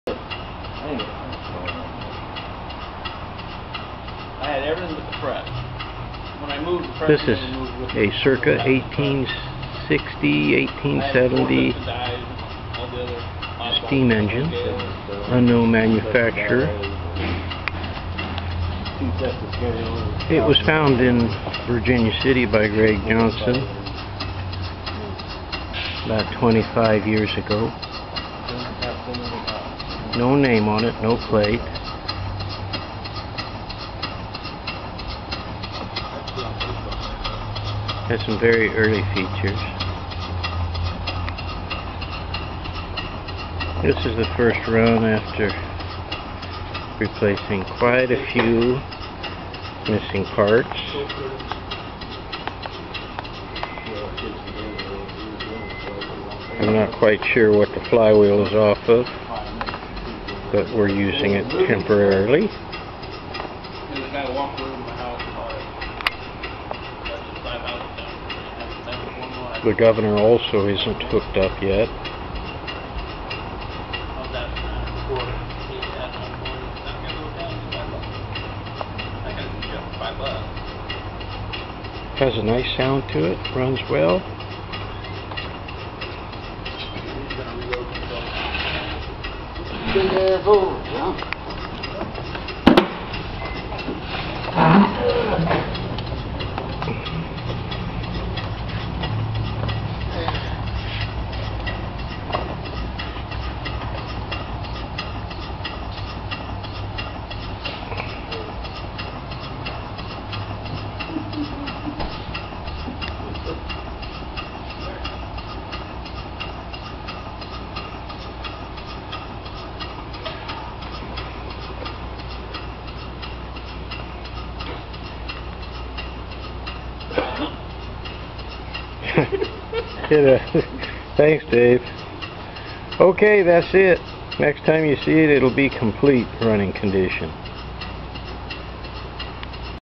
The "Virginia City" Steam engine.
A very early small post Civil war stationary steam engine found near Virginia City Nevada by Greg Johnson about twenty five years ago. Greg rescued it from a mining junk yard seeing the early features and potential to be a beautiful engine.
  After the manufacture of missing parts and rebuilding other parts this is its first run in probably over a hundred years. Lester Bowman has done the primary restoration on this engine with emphasis on creating the missing parts "Era" proper.
  The governor pulleys and belting will be the next project on this engine. Also the machining of correct Oil cups and various other details remain to be done. Lastly the parts will be antiqued to match the original parts and this engine will be finished.
   I need a "period" flywheel for this engine. "S" shaped spokes with a rim about 2 or 3" wide. Thin frail spokes and a hub which will fit a crankshaft a bit bigger than an inch in diameter.
   After it is finished I will post another video. Hope you enjoy this beautiful quiet running long stroke engine.
   Its original builder is not known nor its original purpose. However it probably operated a small single stamp mill or small ore pulverizer  in an Assayers office. It might have operated a sluice box or a pump to supply water in a mining capacity. Whatever its original use, it remains a rare example of early American steam engine retaining some of the earliest features of steam engine design.
   Any information as to whom built this engine would be greatly appreciated. Thank you.